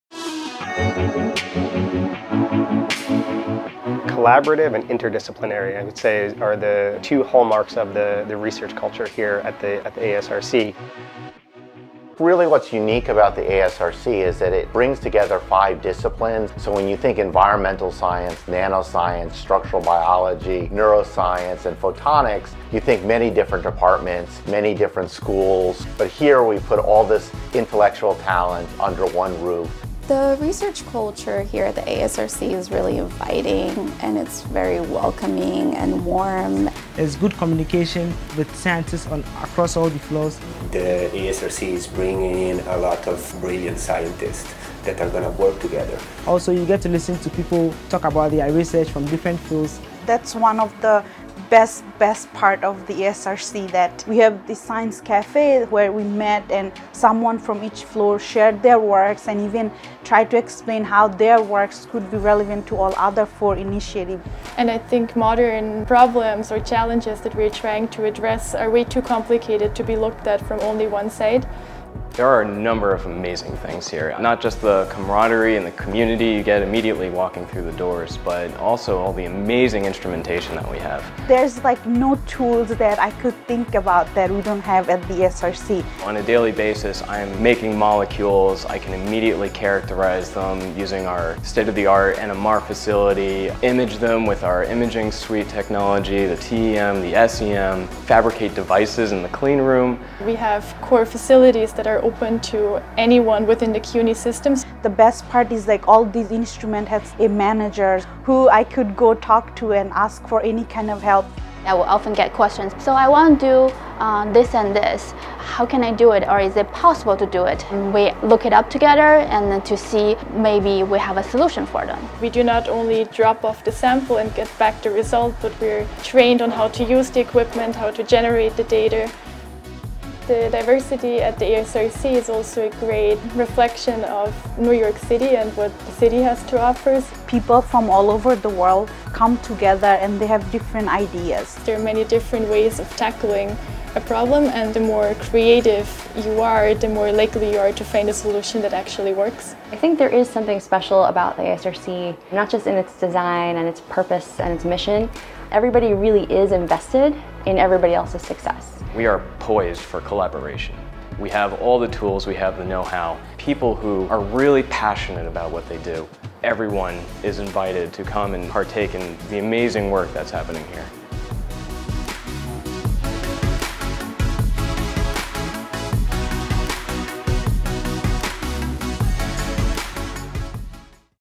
Interdisciplinary Research at the ASRC
Today, scientists are called to address a growing demand for technological advances and an accelerating list of threats to our planet and humanity. The solutions to these challenges will not be realized within siloed areas of research, but through the dynamic, innovative thought produced when scientists work across disciplines for the public good.

Learn how students and faculty at the ASRC are working across five distinctive, but increasingly connected areas of research – environmental science, nanoscience, neuroscience, photonics, and structural biology – to advance creative, multidisciplinary solutions to the most pressing issues facing society.